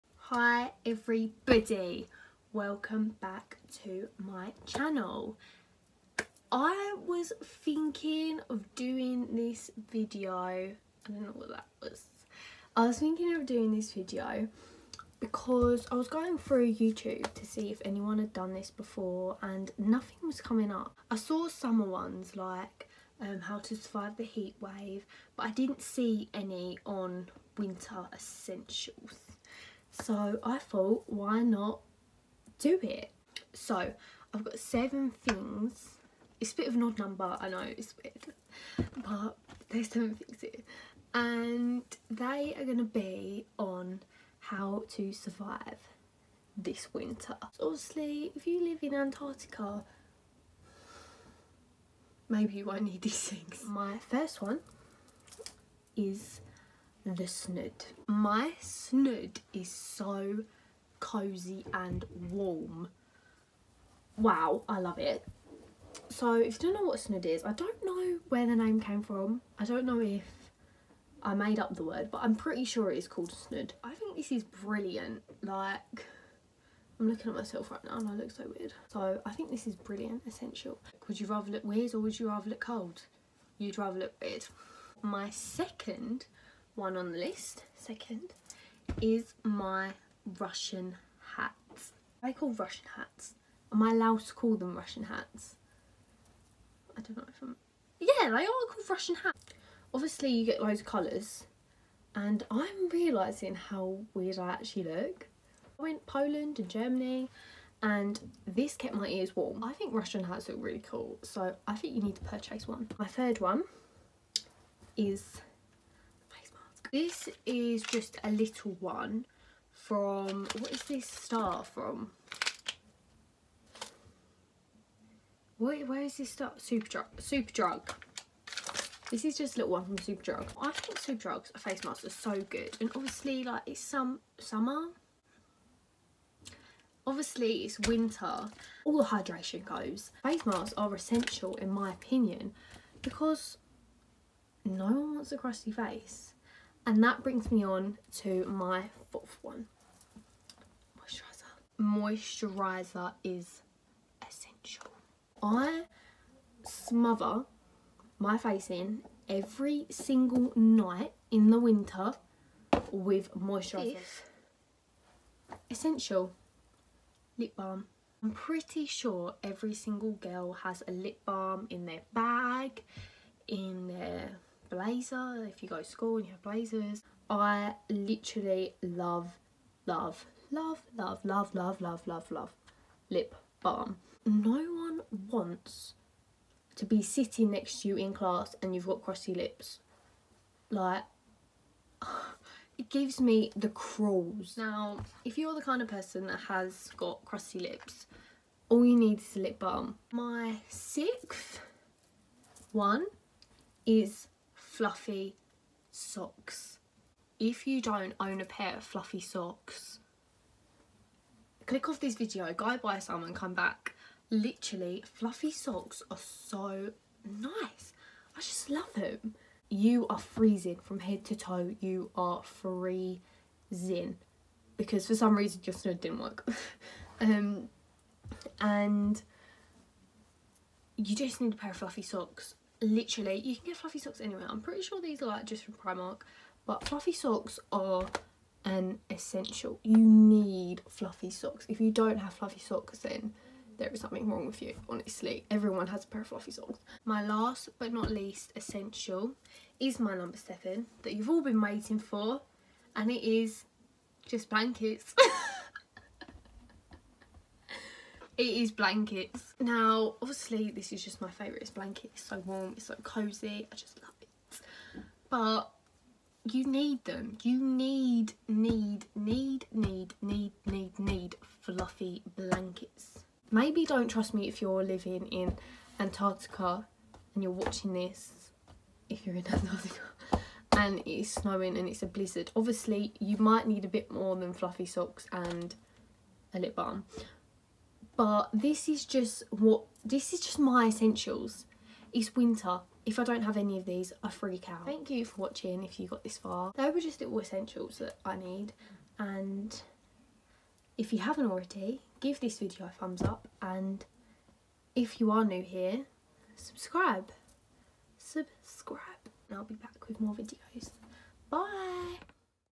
My Winter Essentials!!
where you can find everything in my video:
Snood- Primark
Russian hat- New Look
Face mask (avocado)- Superdrug
Moisturiser (nivea)- Superdrug
Lip balm (carmex)- Superdrug
Fluffy socks- Primark
Blankets- Primark
where else you can find me: